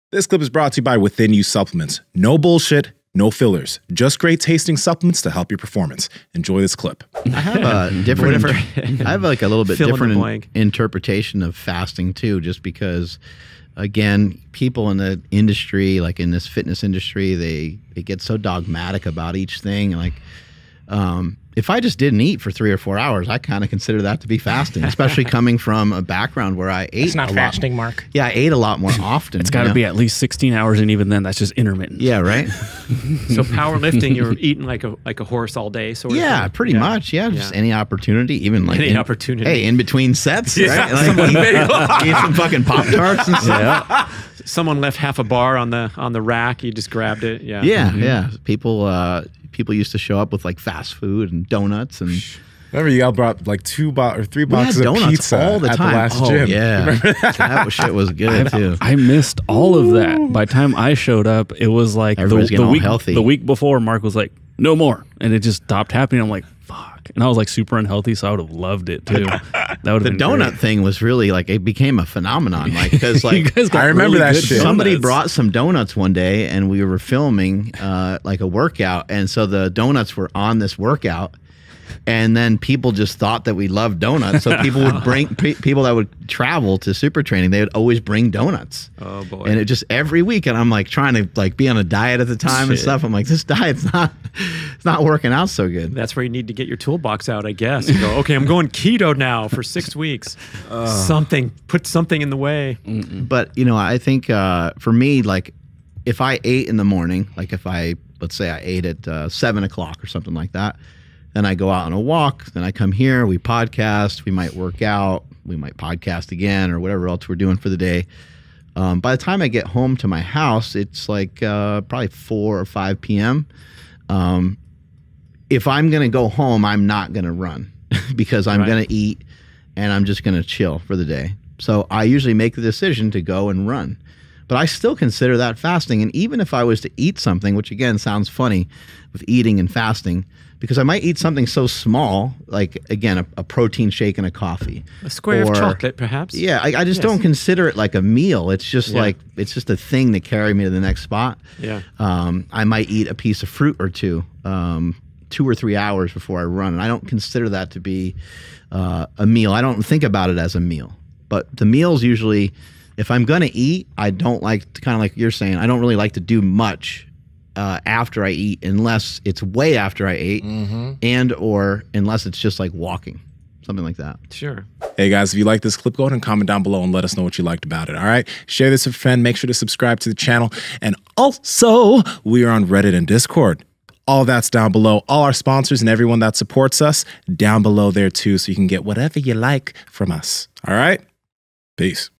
Mark Bell's Interpretation of Fasting
In this clip, Mark Bell, Nsima Inyang, Brad Kearns and Andrew Zaragoza talk about fasting